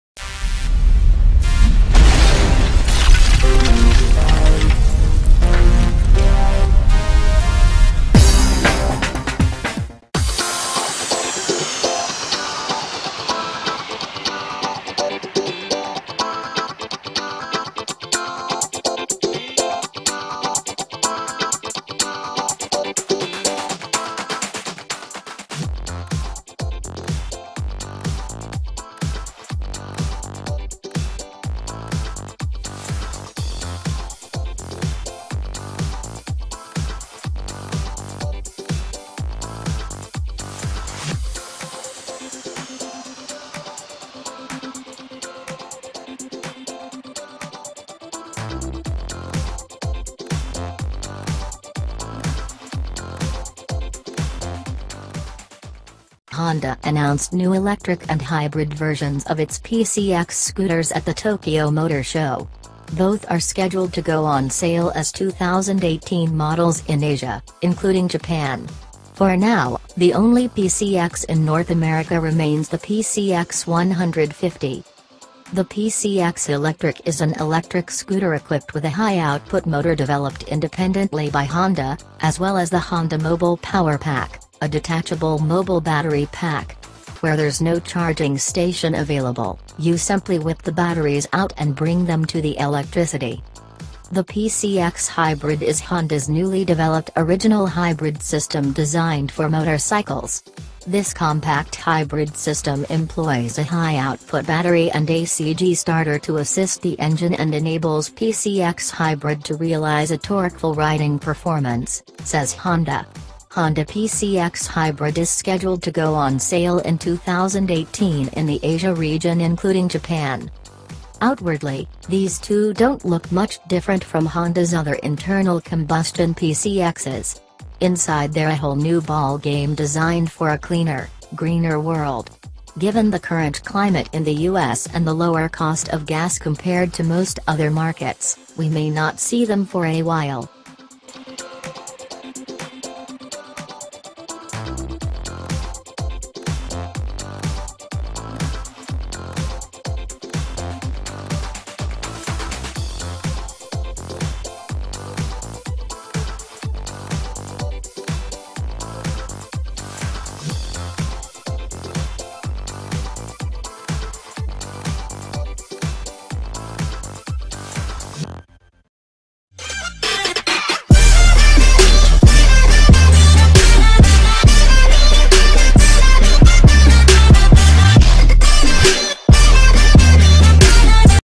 Honda PCX Scooter Goes Hybrid and Electric | Tokyo Motor Show 2018
Honda announced new electric and hybrid versions of its PCX scooters at the Tokyo Motor Show. Both are scheduled to go on sale as 2018 models in Asia, including Japan. For now, the only PCX in North America remains the PCX 150.

The PCX ELECTRIC is an electric scooter equipped with a high-output motor developed independently by Honda, as well as the Honda Mobile Power Pack, a detachable mobile battery pack. Where there’s no charging station available, you simply whip the batteries out and bring them to the electricity.

The PCX HYBRID is Honda’s newly-developed original hybrid system designed for motorcycles. “This compact hybrid system employs a high-output battery and ACG starter to assist the engine and enables PCX Hybrid to realize a ‘torqueful’ riding performance,” says Honda. Honda PCX Hybrid is scheduled to go on sale in 2018 in the Asia region including Japan.